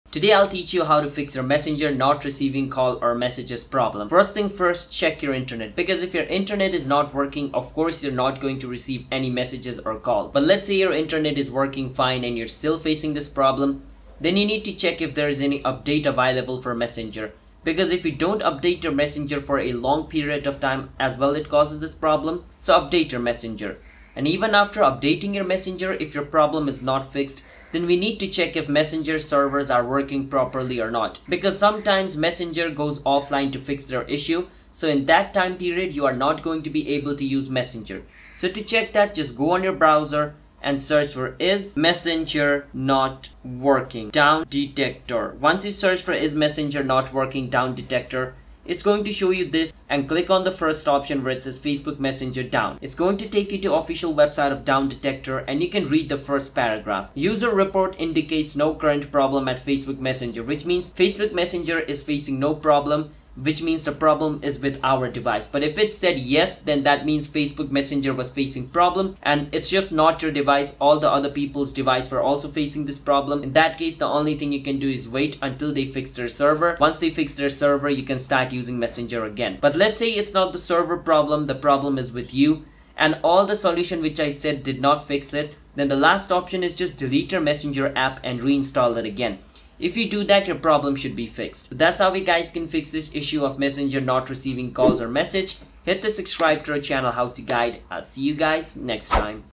FIX Messenger Not Receiving Calls Or Messages (Quick & Easy!)
Confused about how to FIX Messenger Not Receiving Calls on iphone? Are you having trouble receiving calls or messages on Facebook Messenger? You're not alone! This is a common problem, but there are a few things you can do to fix it.